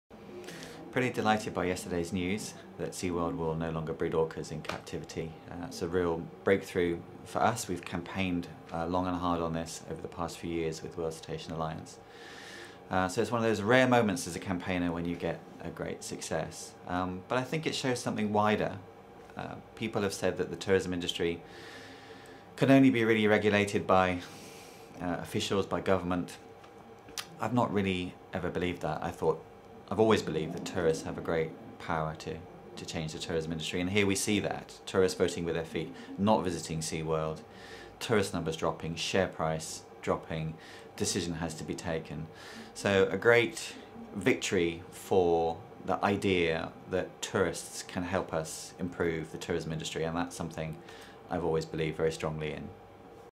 Justin Francis - Responsible Travel response to SeaWorld ending orca breeding
Justin Francis, CEO Responsible Travel discusses the power of tourists to change the tourismi industry in response to SeaWorld announcement that it will be stopping orca breeding campaign